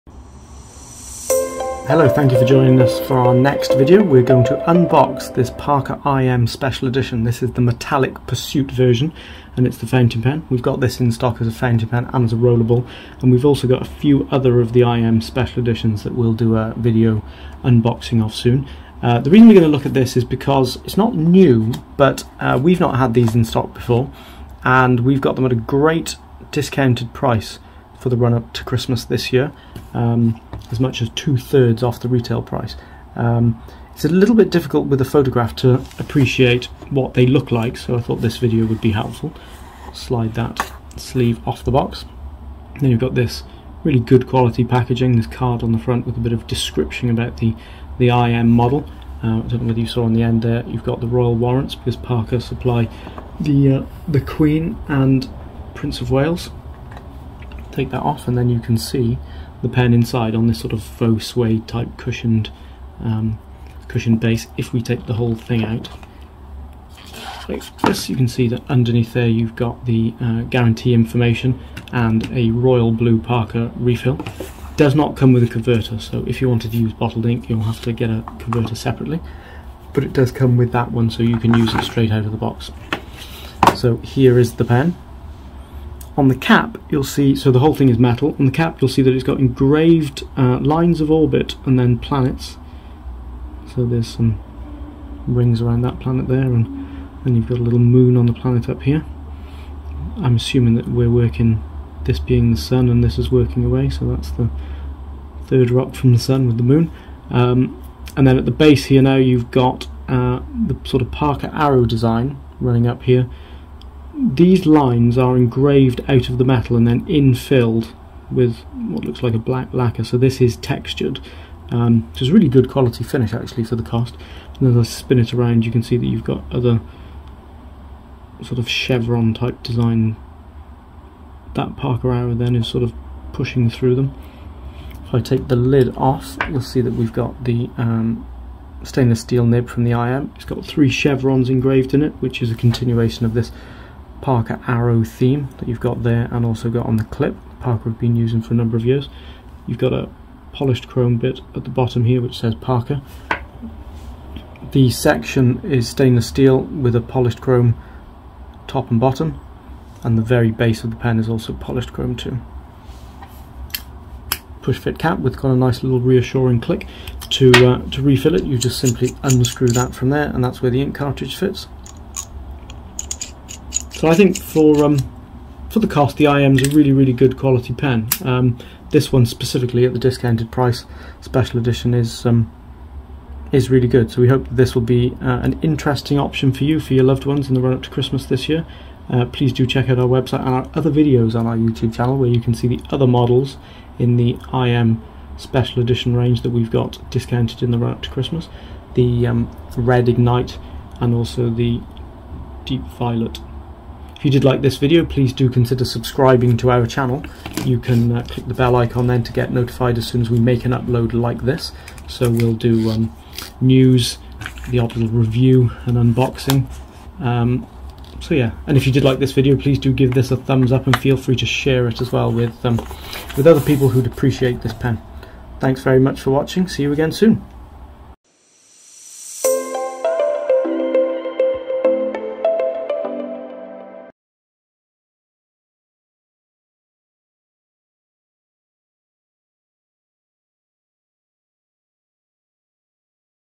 Unboxing the Parker IM Metallic Pursuit Special Edition Fountain Pen
Unboxing the Parker IM Metallic Pursuit Special Edition Fountain Pen from Pure Pens. It's available now at a great price for Christmas 2021, whilst stock last!